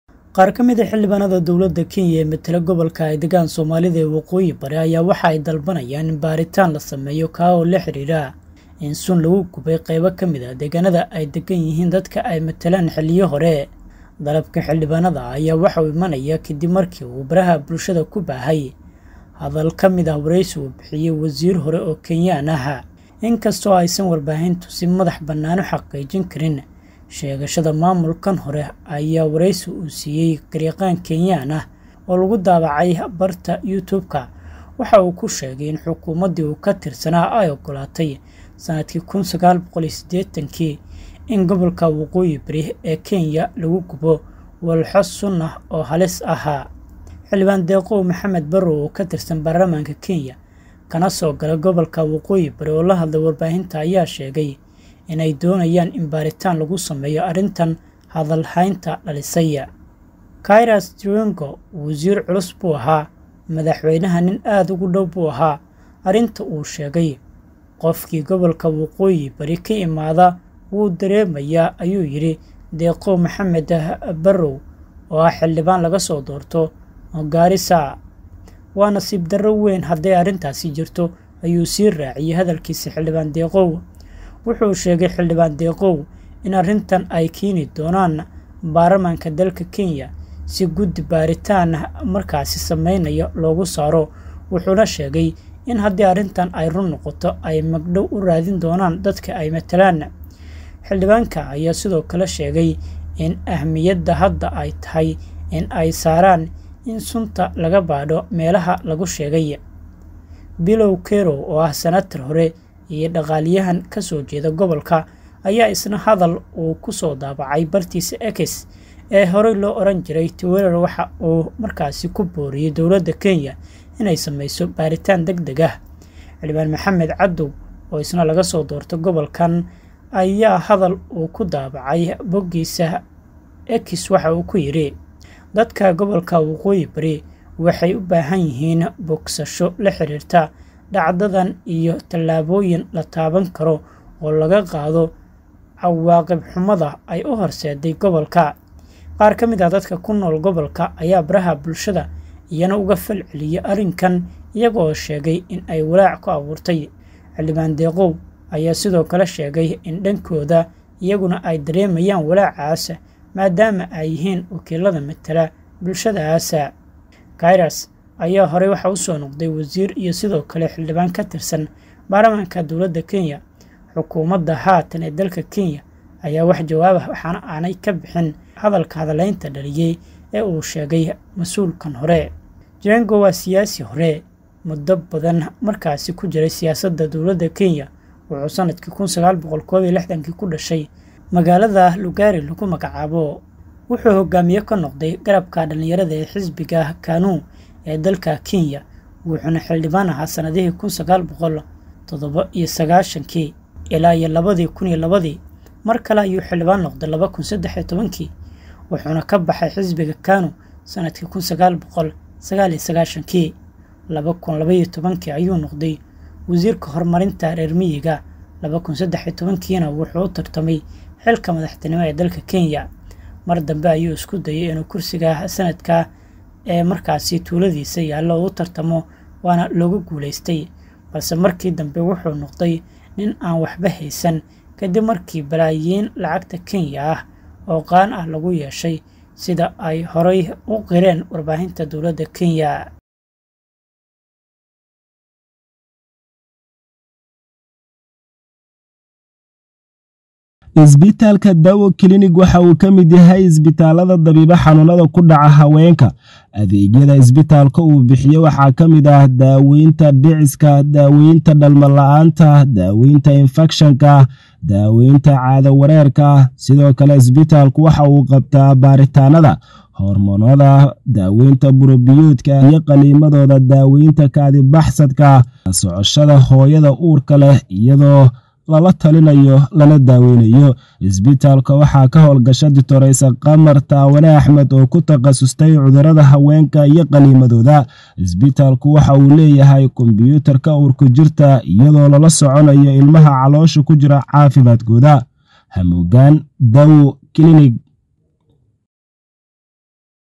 Muxuu yahay warka walaaca ku abuuray qaar kamid ah xildhibaanada gobolka Soomaalida ee Kenya?
Muxuu yahay warka walaaca ku abuuray qaar kamid ah xildhibaanada gobolka Soomaalida ee Kenya ee hadal haynta dhaliyay?